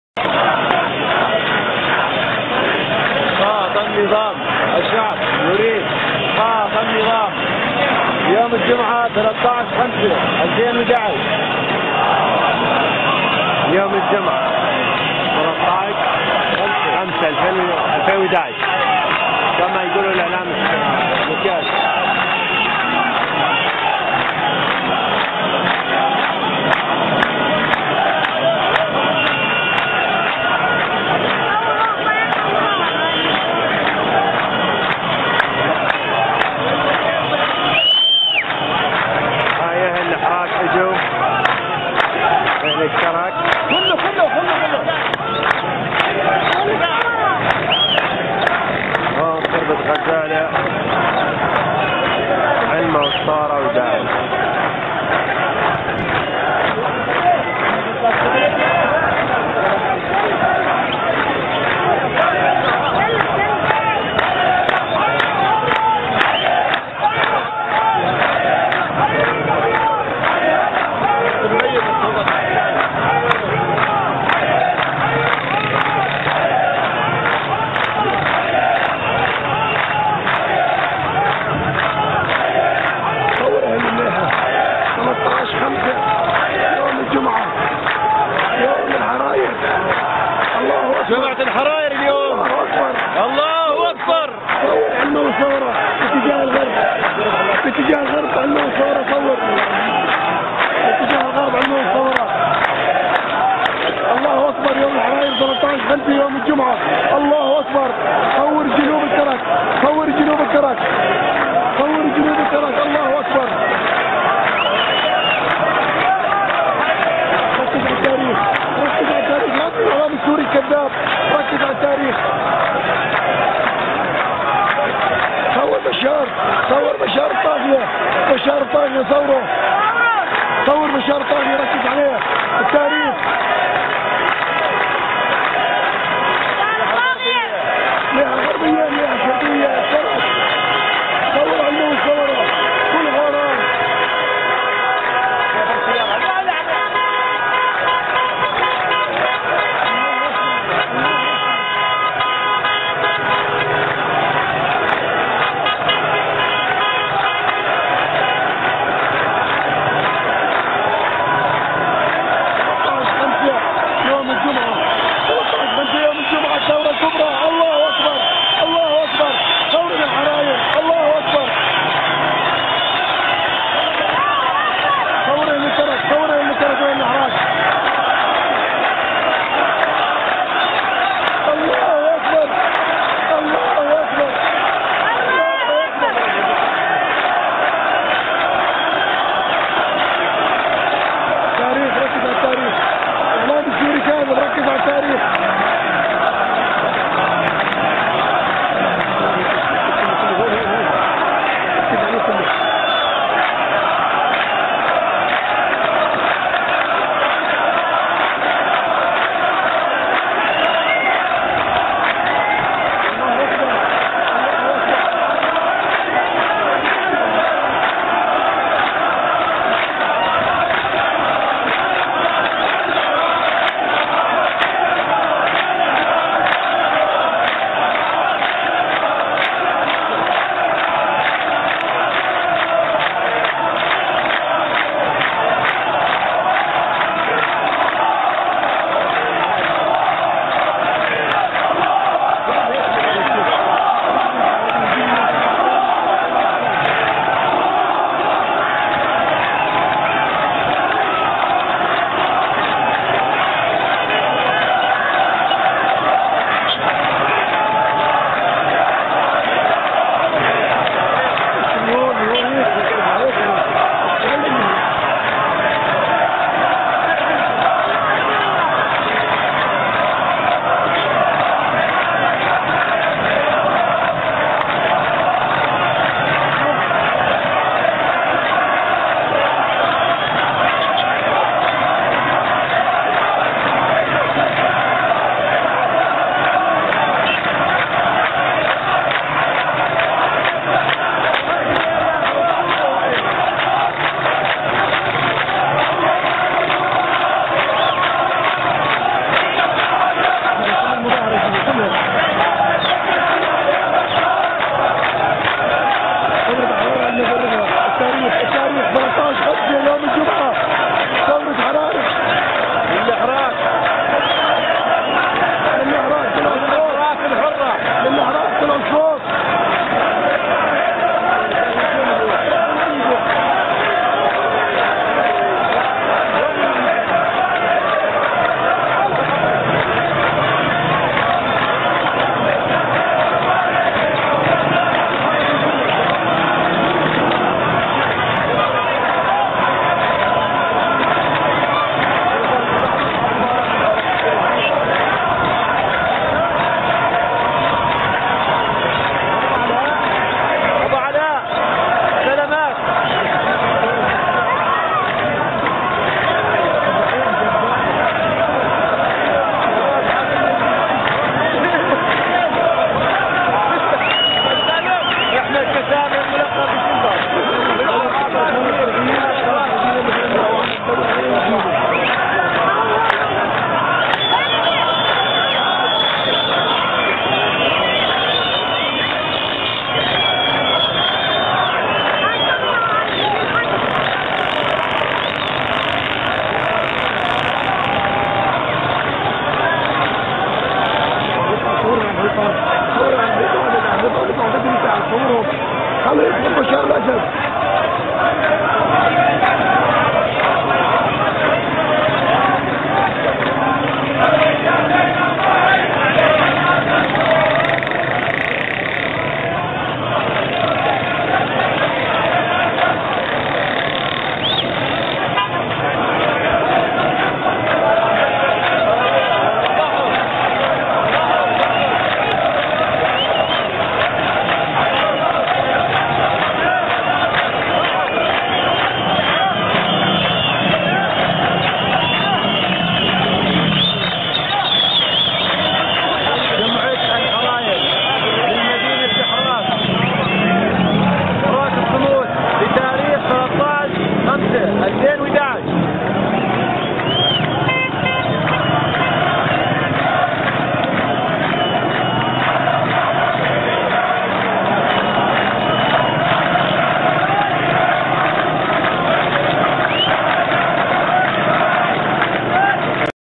Huge demos in Daraa city in spite of the suppression of the bloody regime in Syria 13.05.2011
Huge demonstrations in Daraa city in spite of the suppression of the bloody regime in Syria 13.05.11

It is the only way to show the world what is happening in Syria since there are no CNN, BBC, Sky news ... etc.